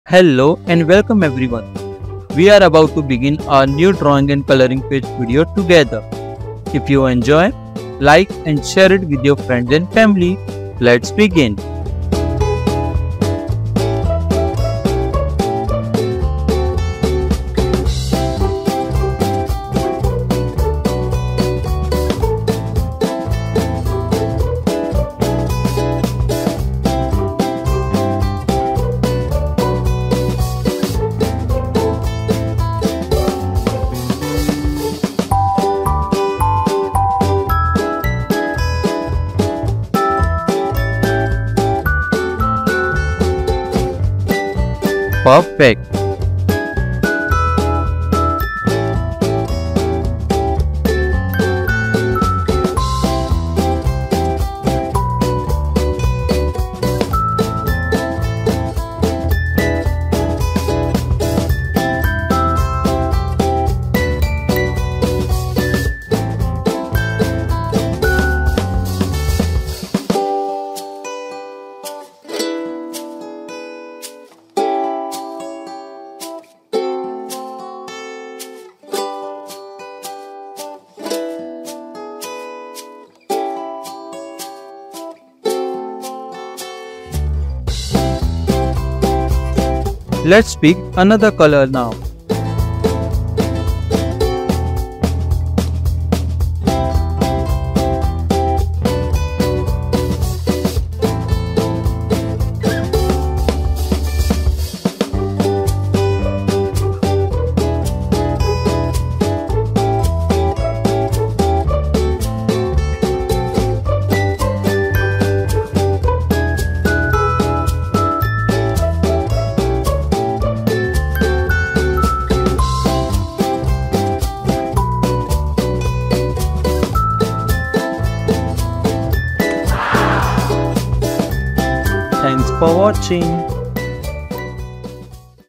🍄 Enchanted Forest! Coloring a Magical Mushroom 🌳
Join me on this fantastical journey! Today, we're coloring a mystical mushroom in an enchanted forest, using vibrant colors and creative techniques. Perfect for kids and teenagers who love art, nature, and fantasy. Let's add some magical details to this whimsical mushroom!